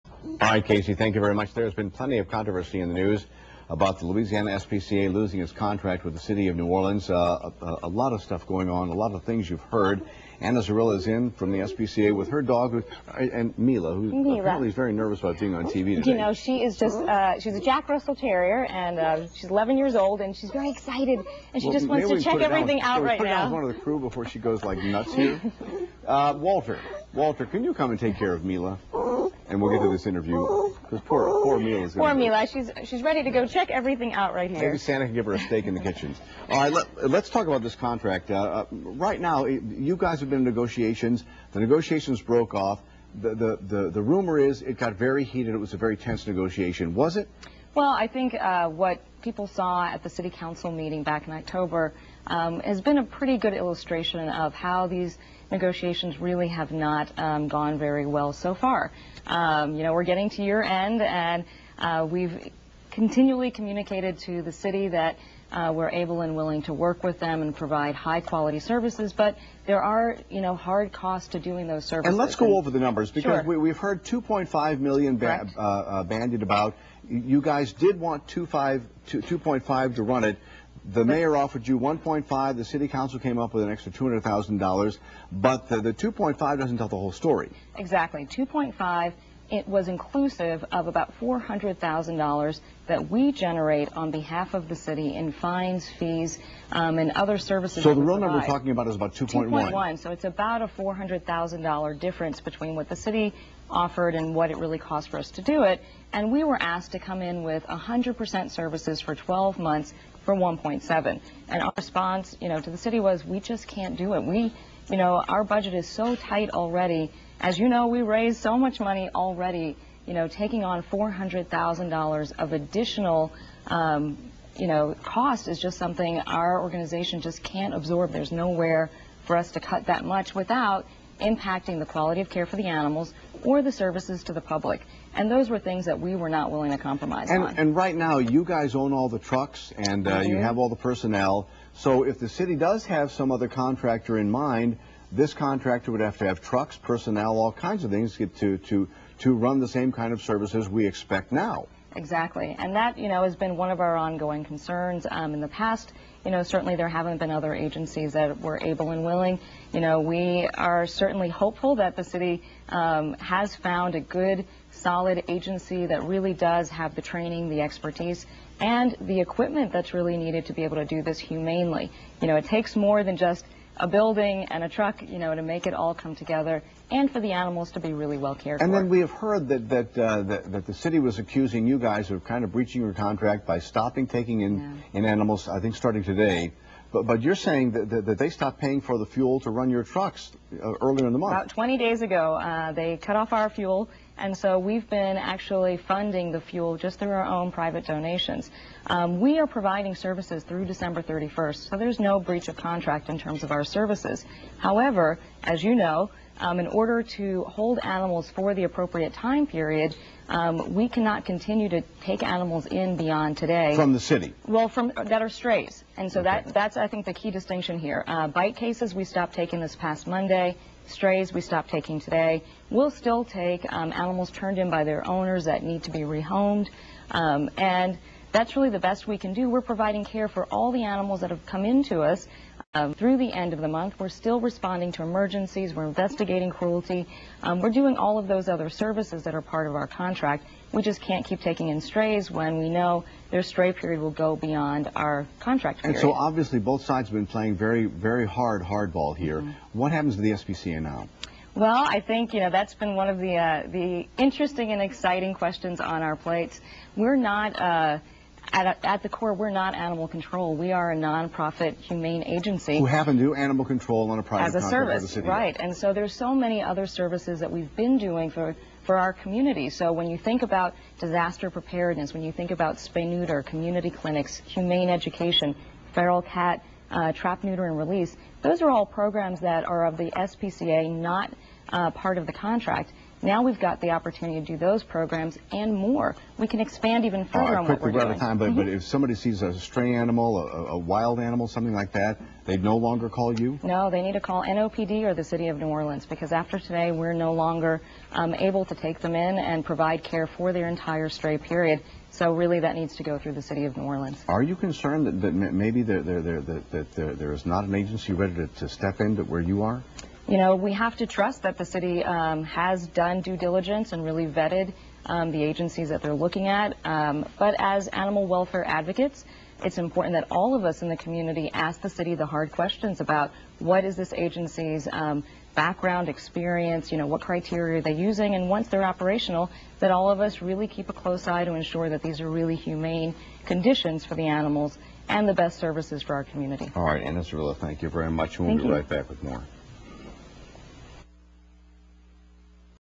Ana Zorrilla and Eric Paulson Discuss Animal Control for New Orleans
Ana and Eric discuss LA/SPCA's position in regards to Animal Control for New Orleans.